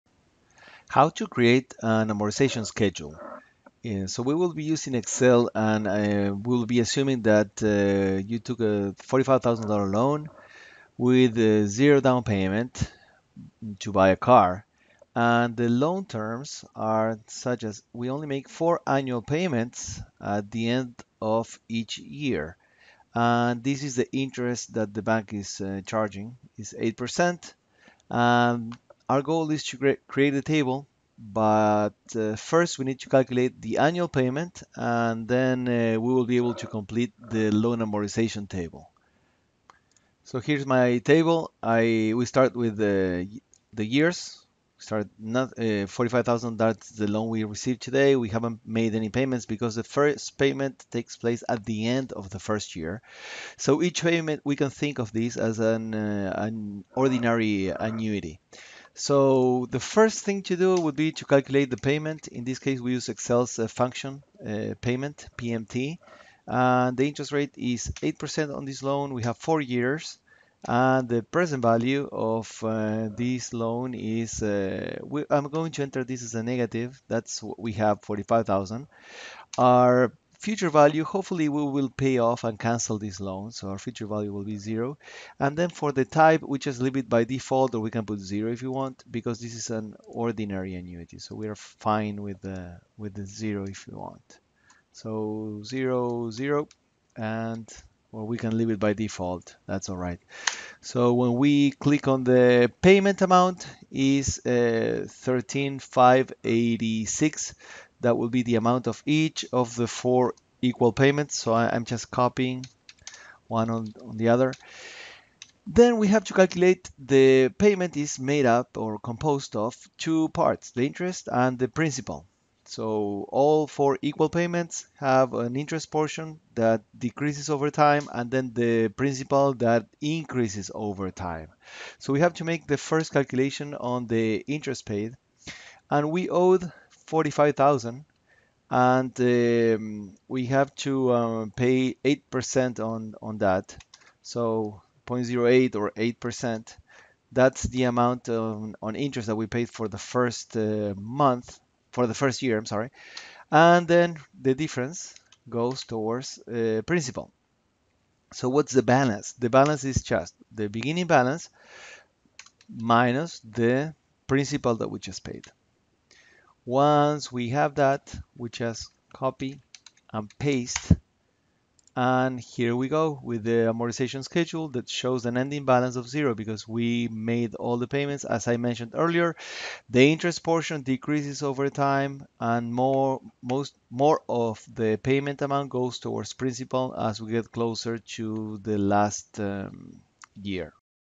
Loan amortization table in Excel
How to calculate the loan payment and complete a loan amortization table using Excel.